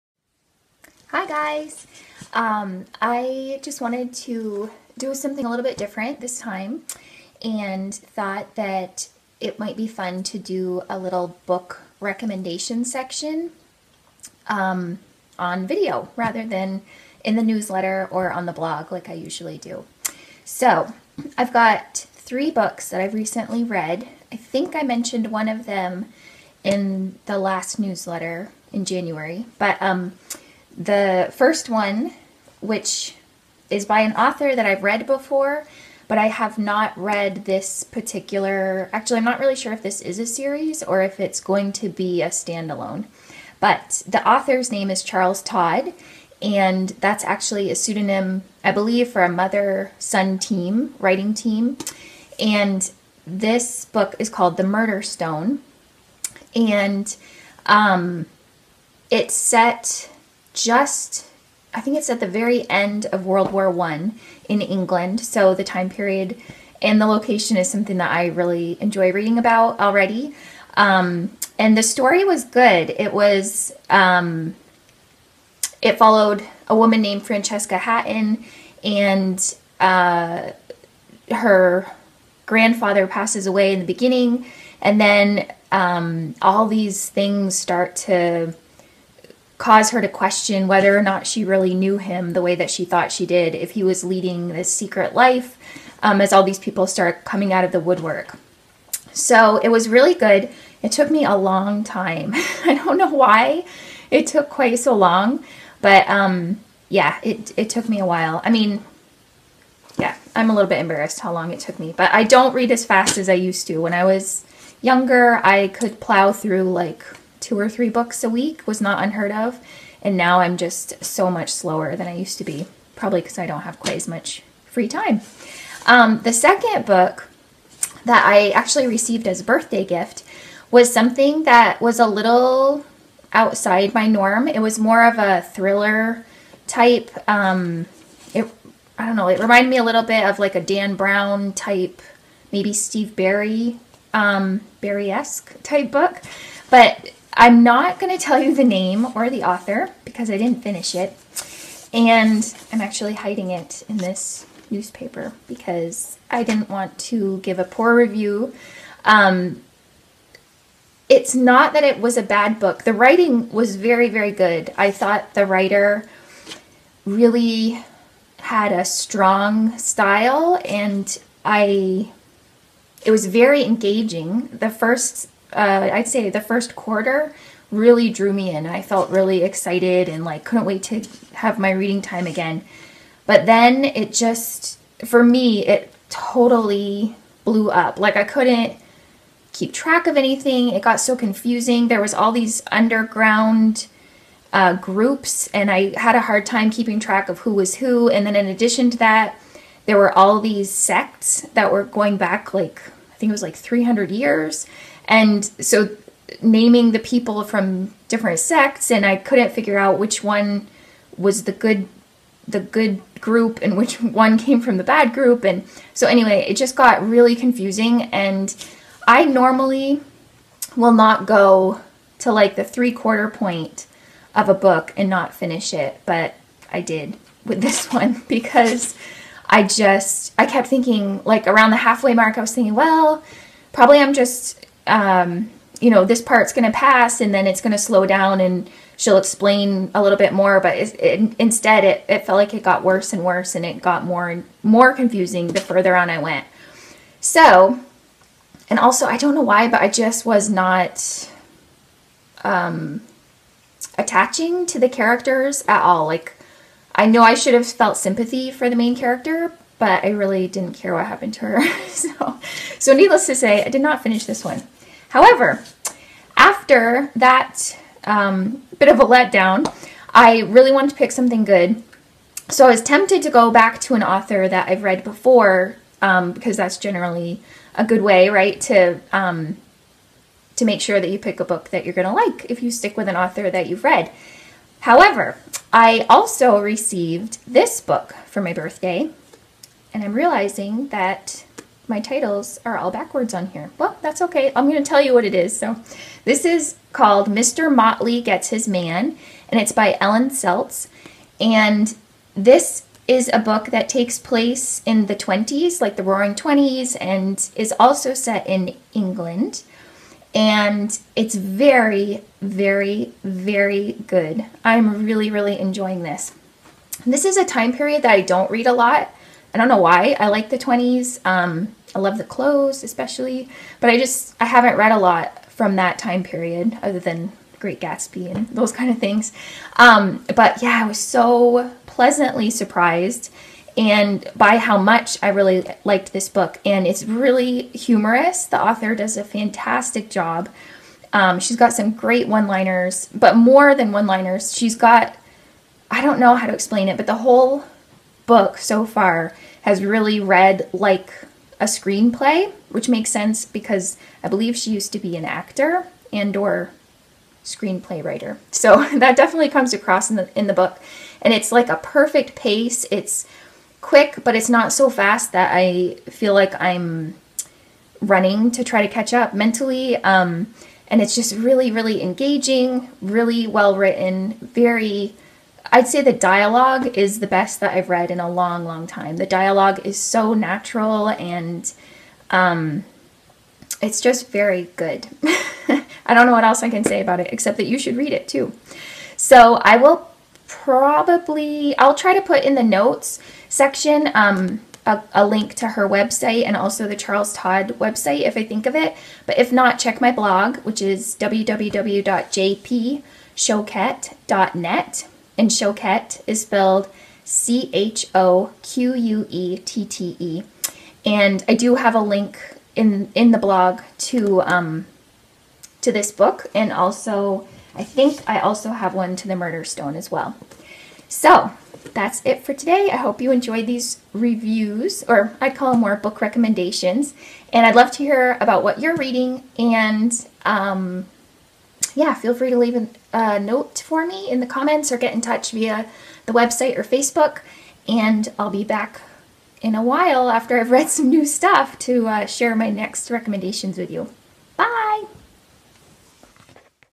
Thriller Book Recommendations: Charles Todd & Ellen Seltz
Excellent reads this month and wanted to recommend them to you. While both fall more under the "mystery book" umbrella, they were both very enjoyable reads. Tune in and see if you agree!